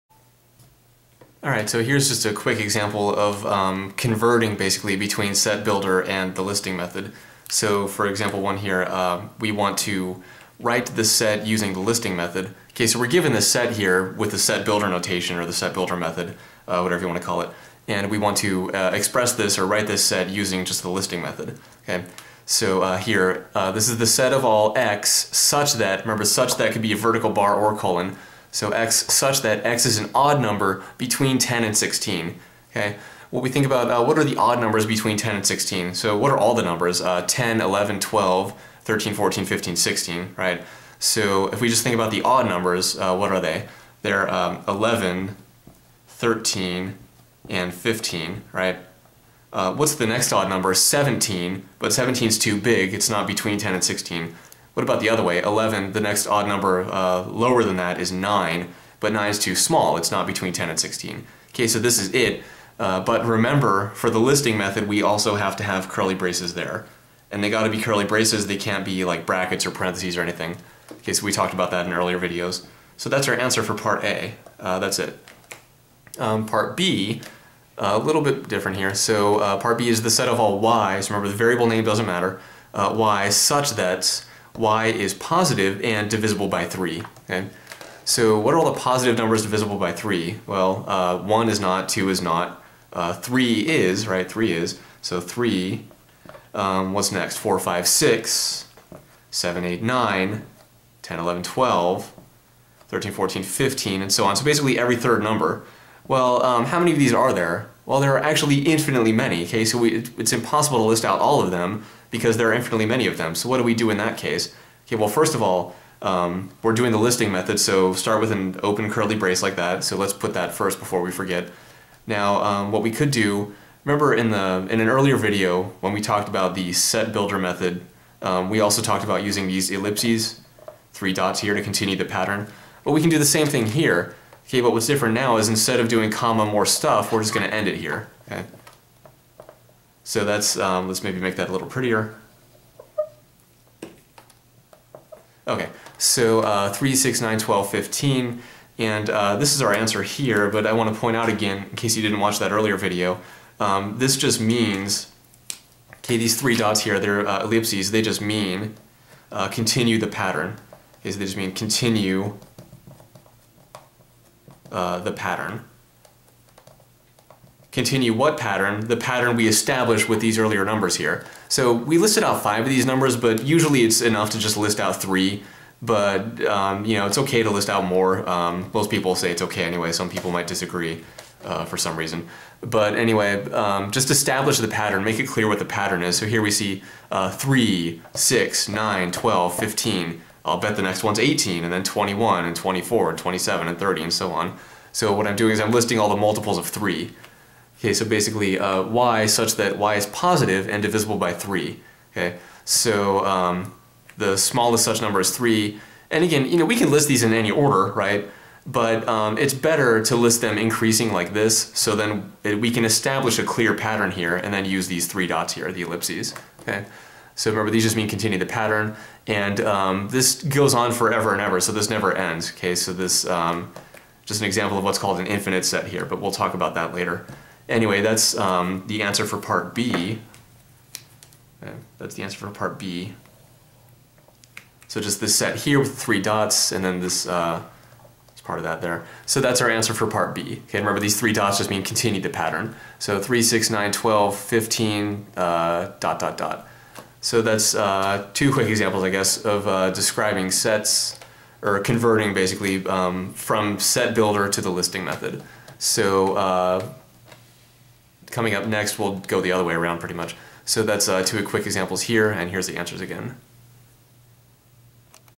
Intermediate Algebra - Describing Sets - Example 1
An example of converting from set builder notation to the listing method.